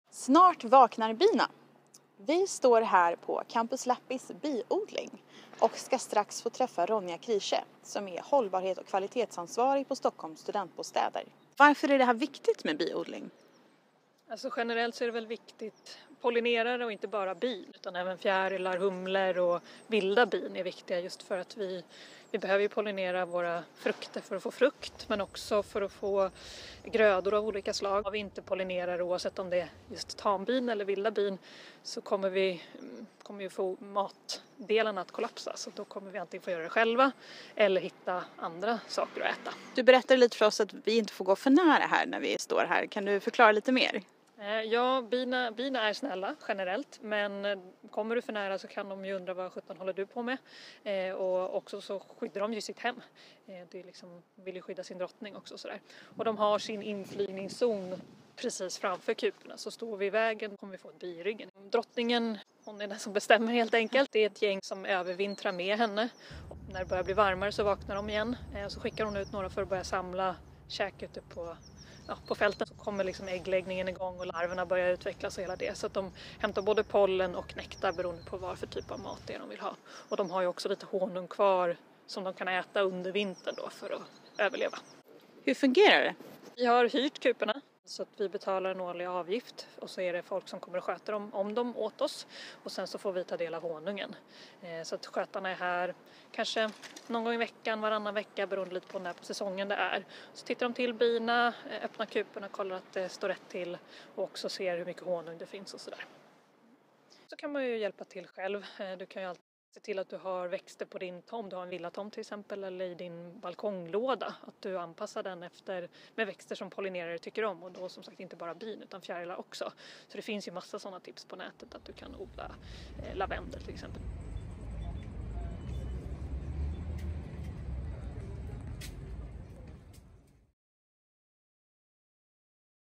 Om blommor och bin
Nu vaknar bina! Läget har besökt Campus Lappis där det finns bikupor sedan ett år tillbaka. Där får vi höra om varför bin är så viktiga för miljön, varför binas överlevnad påverkar vad vi kan äta och vad vi kan göra för att hjälpa våra bin.